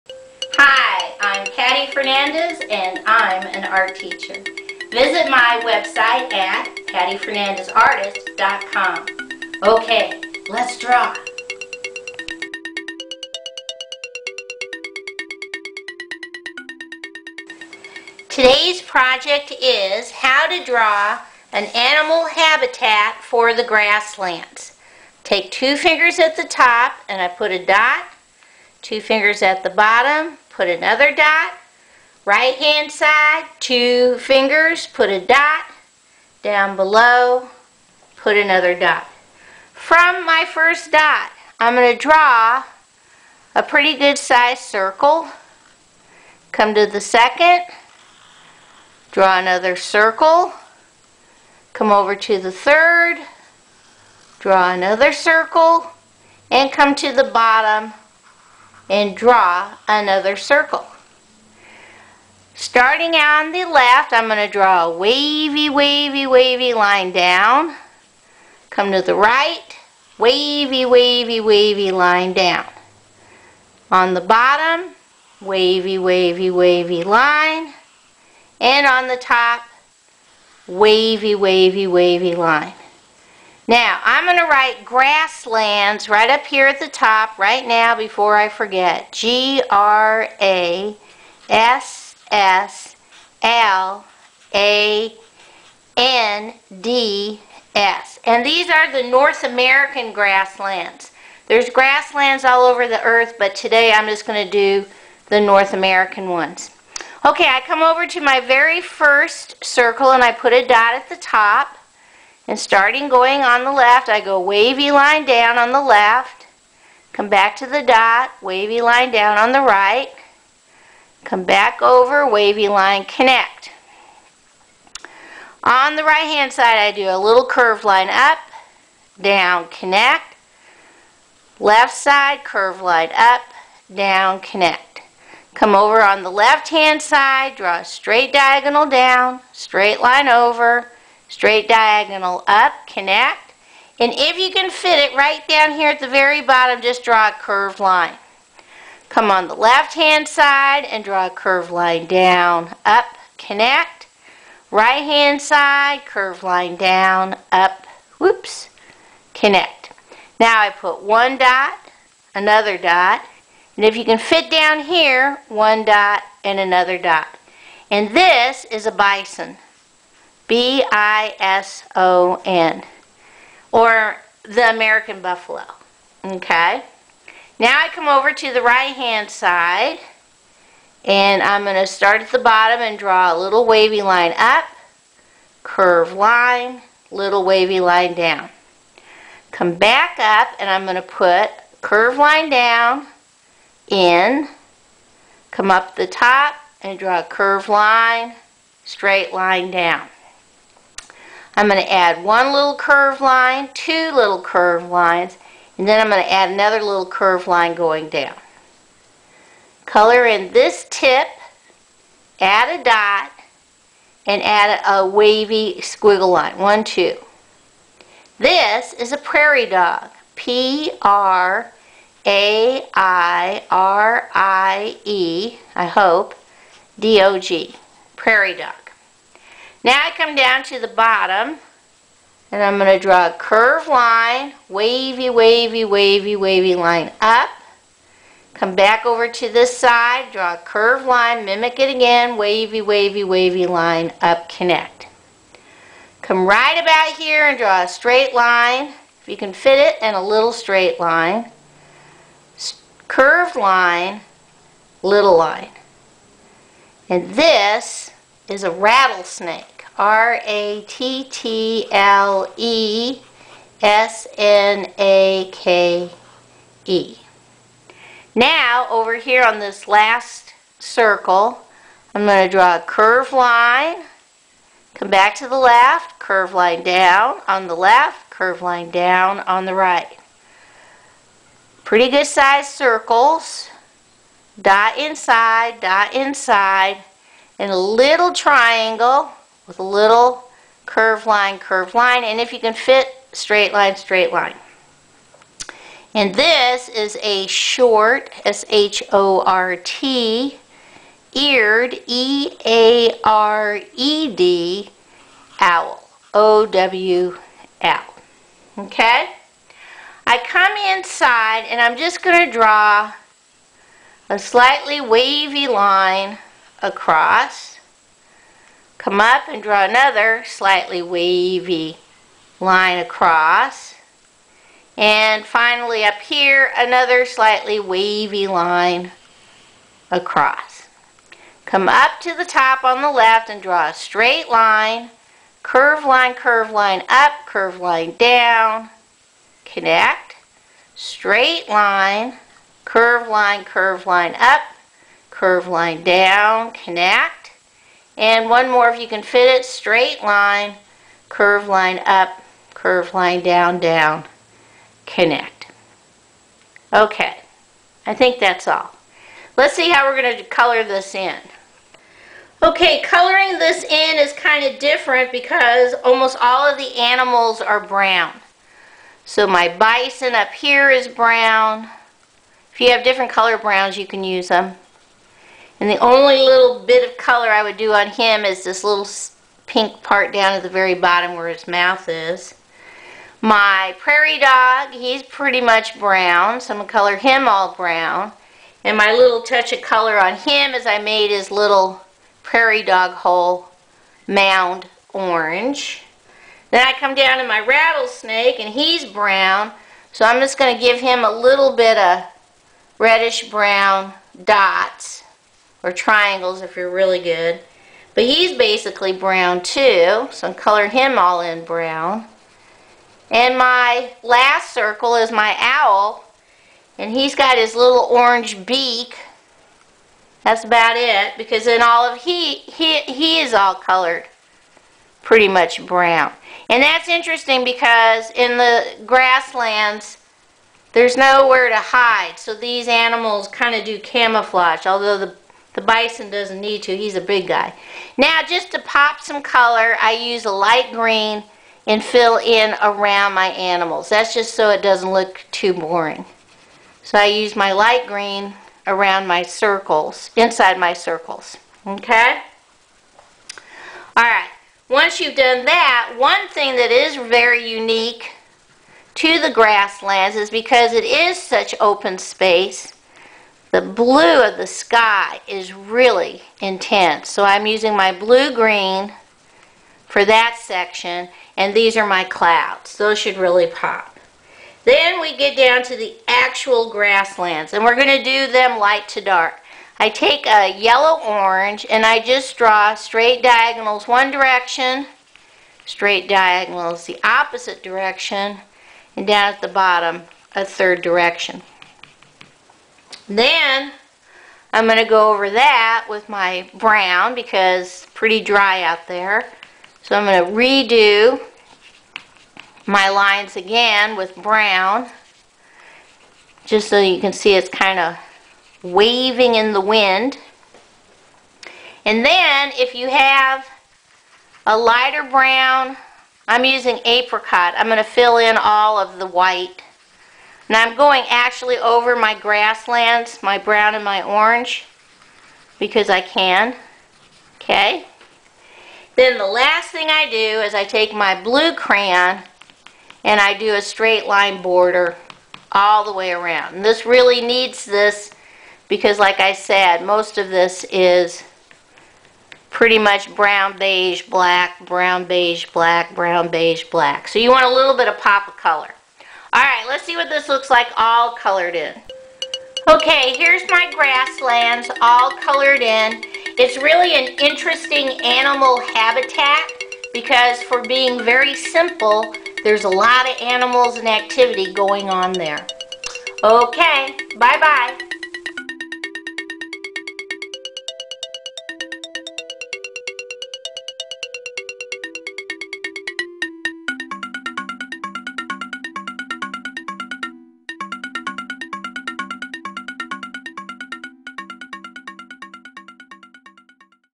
Let's Draw an Animal Habitat-Grasslands
Easy to follow directions, using right brain drawing techniques, showing how to draw an animal habitat-the grasslands.